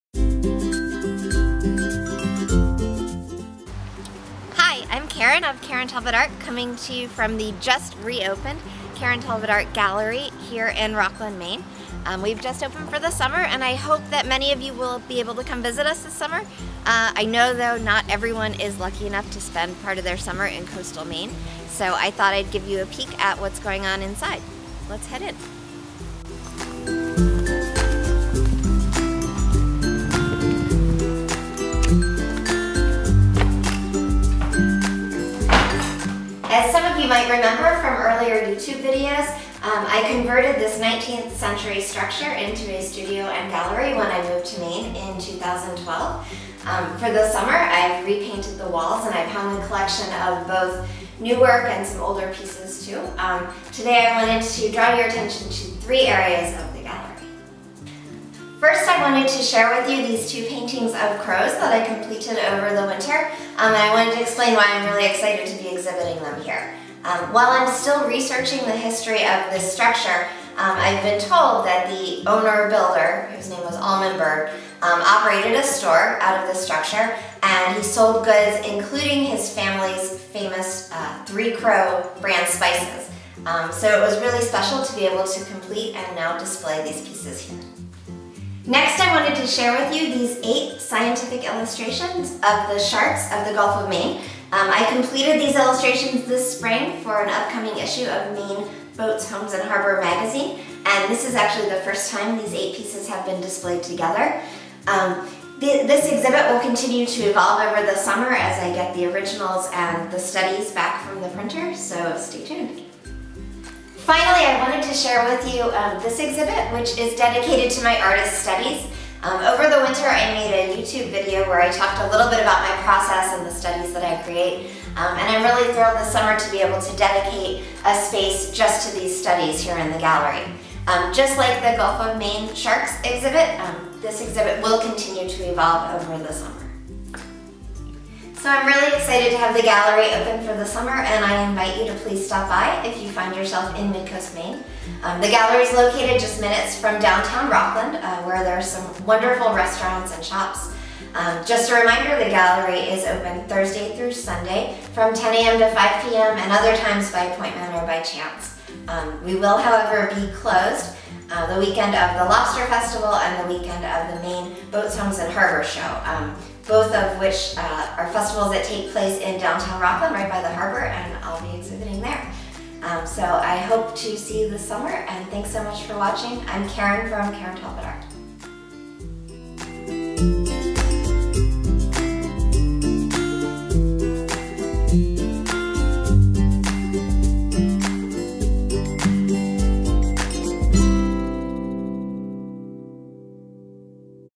2015 Karen Talbot Art Gallery Opening
On June 20, 2015, the Karen Talbot Art Gallery in Rockland, Maine opened for the summer season. In this video, Karen shares three highlights of the Gallery: 1) original crow paintings, 2) Gulf of Maine sharks exhibit and 3) artist's studies exhibit.